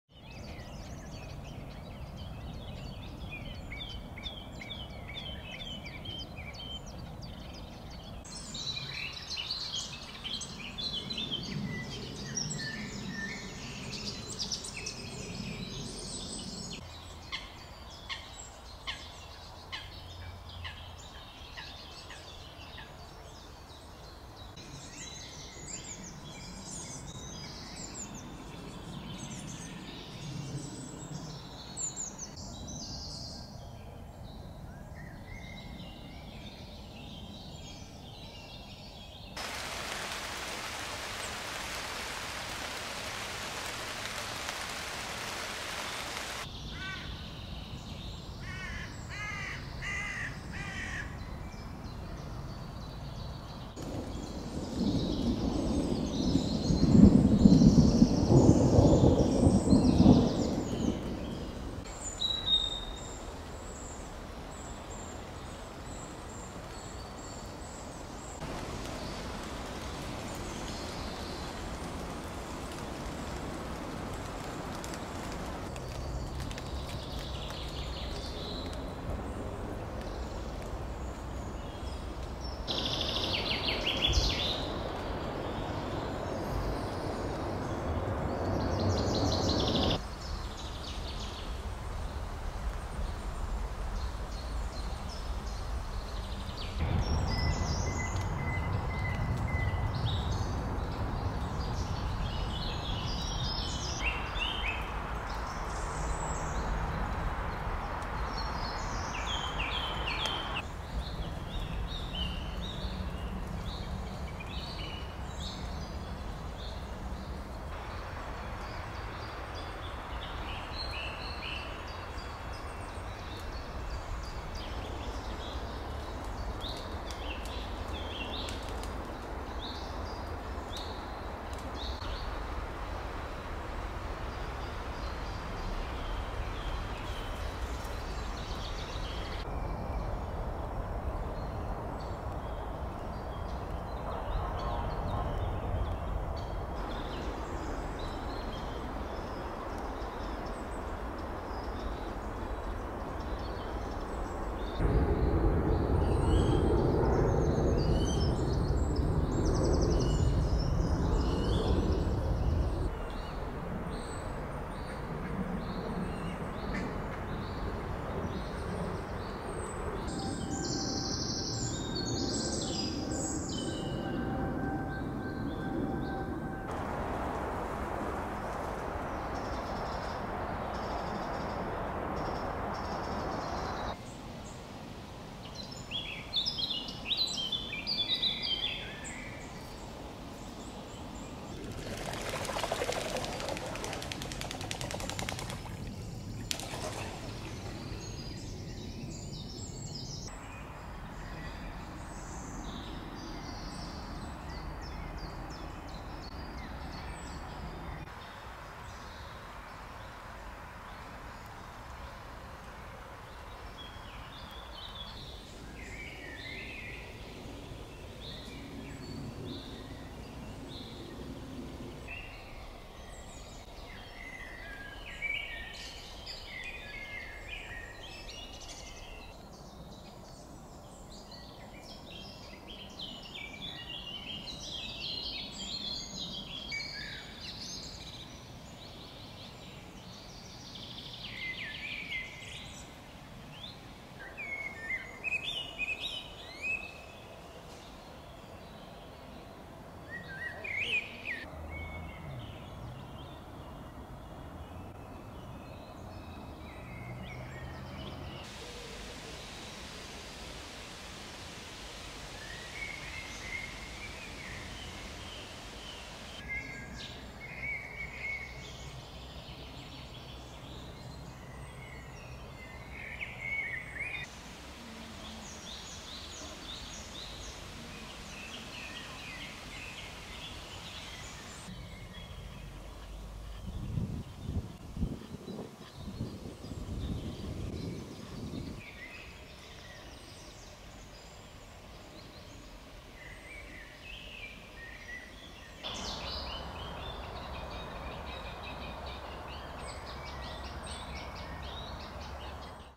Amsterdamse Bos 310522
Een buiige dag met diepe schaduwen, bloeiende vlier en gele lis.